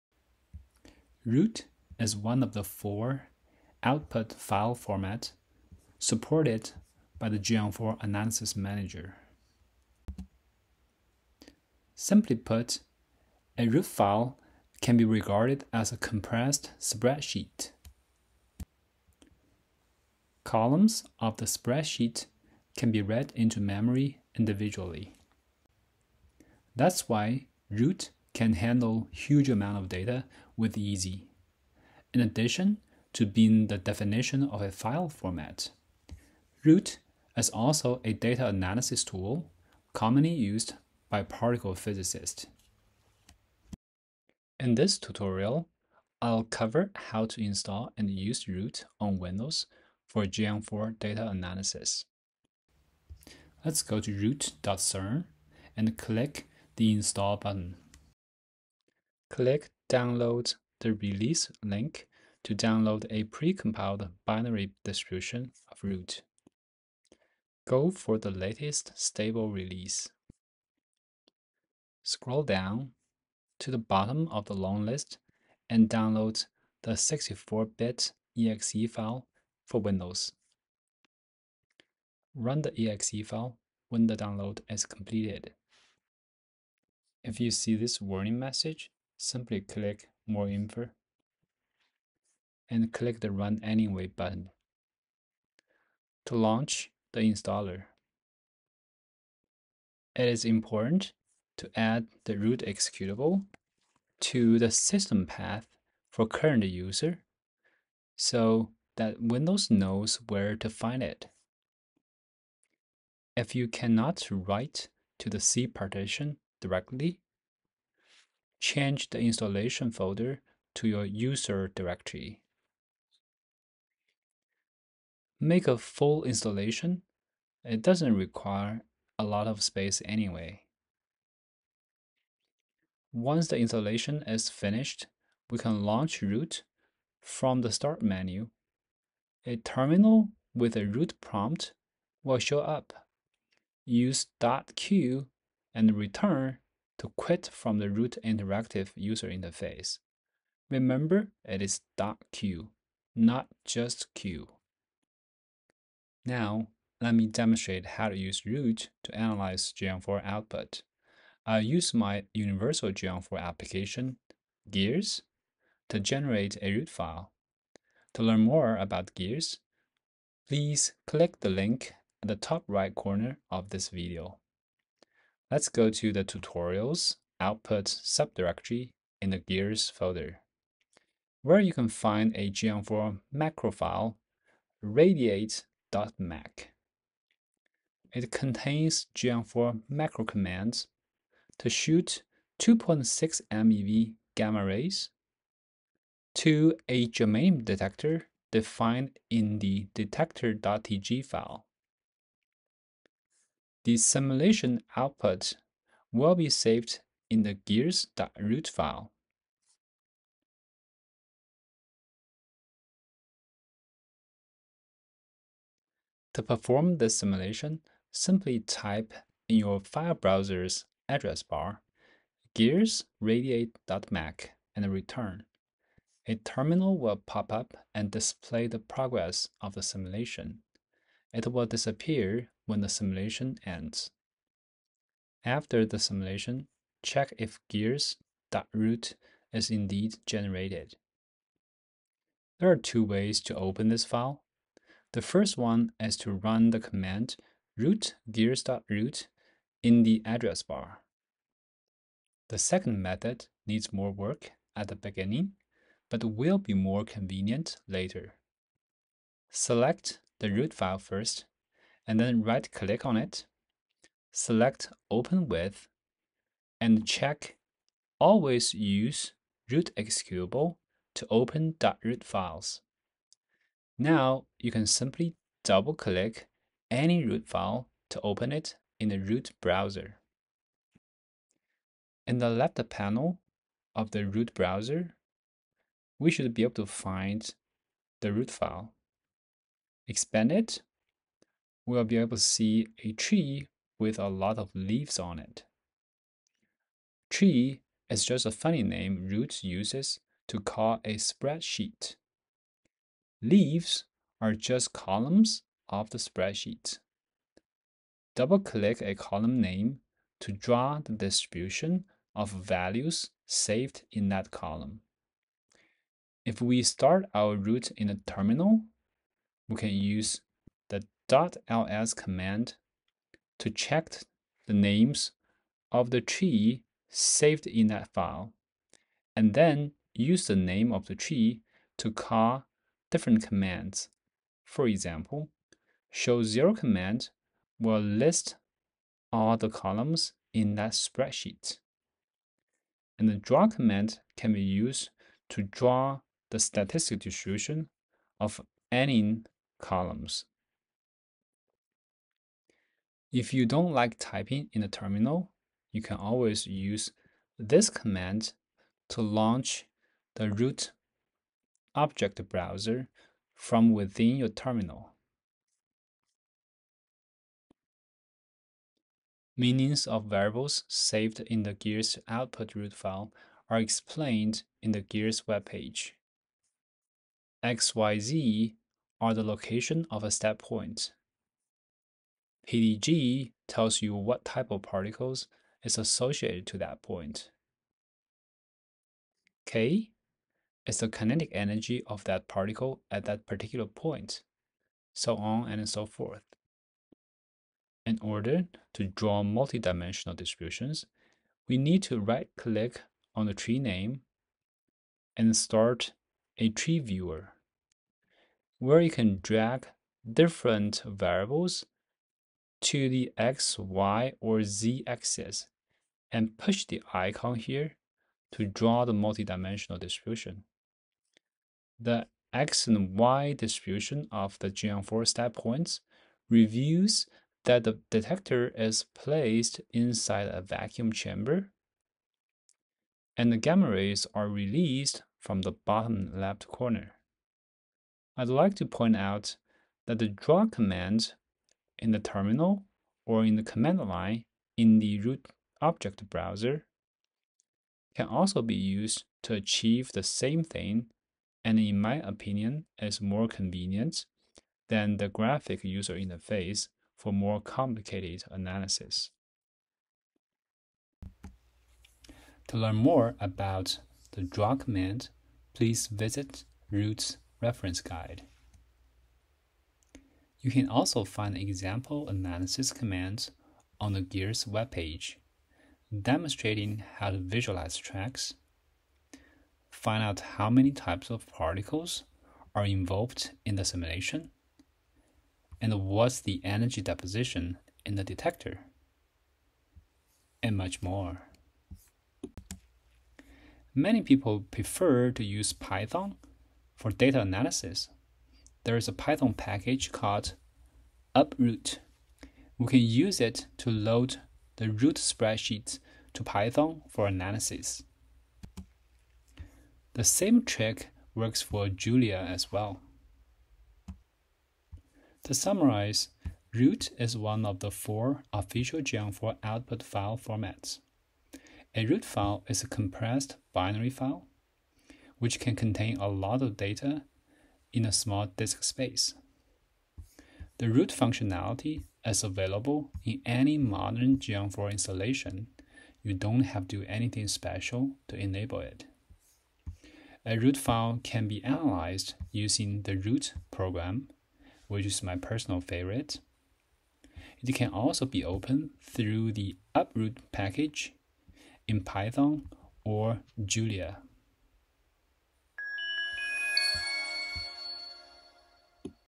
and Use ROOT for Geant4 Data Analysis on Windows
Geant4 output data can be saved in 4 different formats. ".root" file is one of them. This tutorial shows how to install the analysis program to open a root file generated by a Geant4 application and to visualize data saved inside.